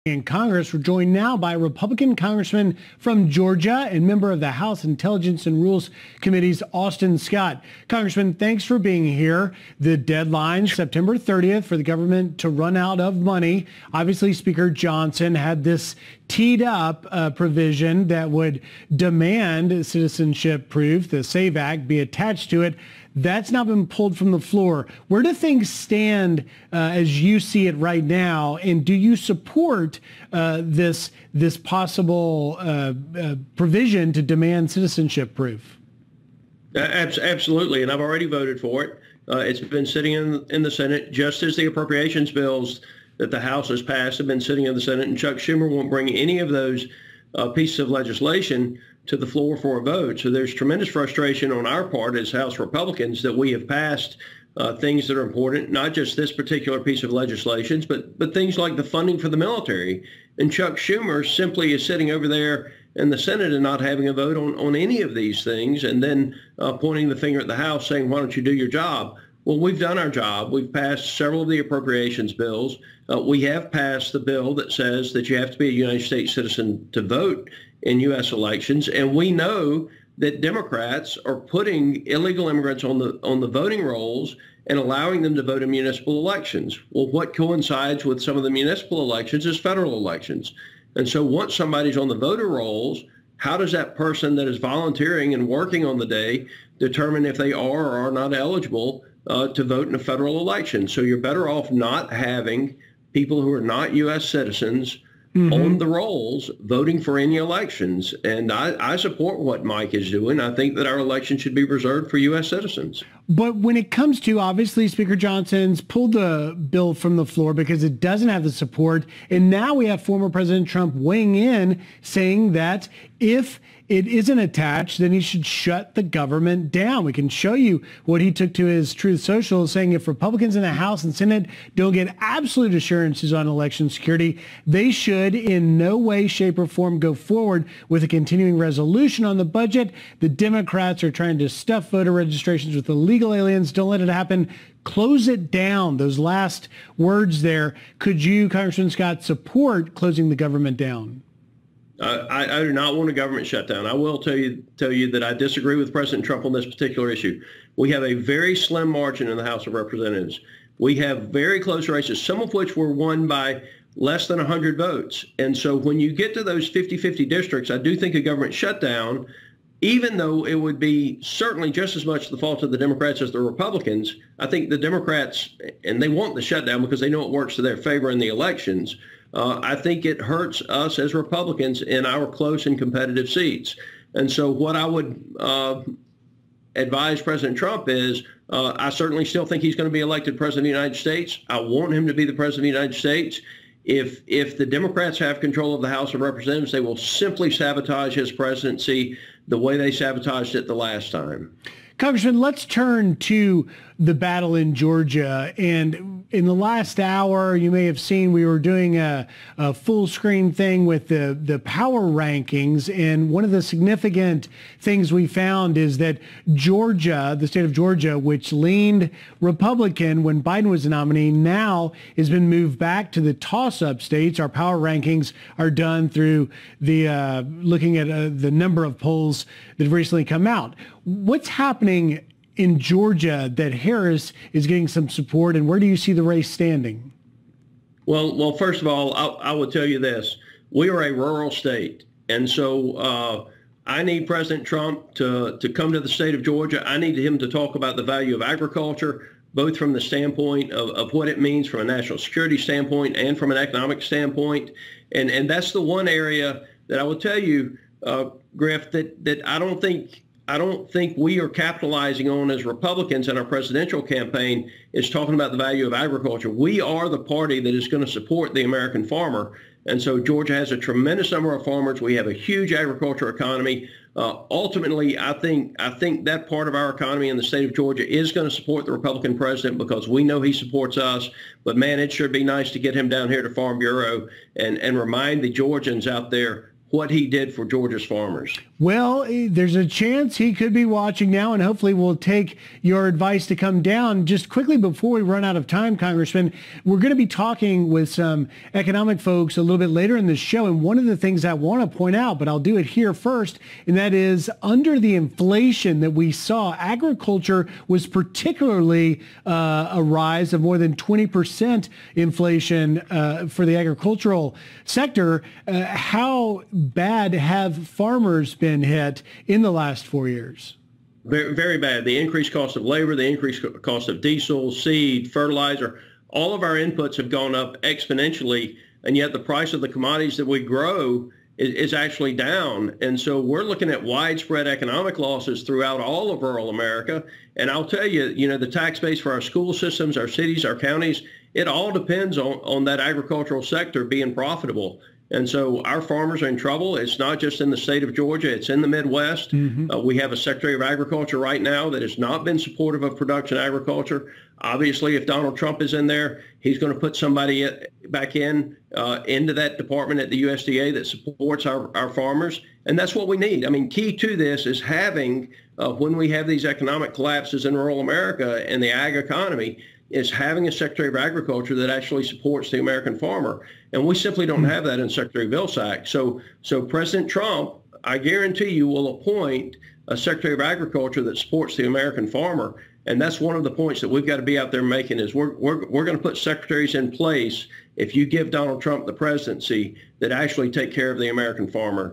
Rep. Austin Scott Joins Fox News to Discuss Biden-Harris Admin's Negative Impact on Rural America
Our farmers have suffered from record inflation and input costs under the Biden-Harris Administration.

This has caused widespread economic loss across all of rural America.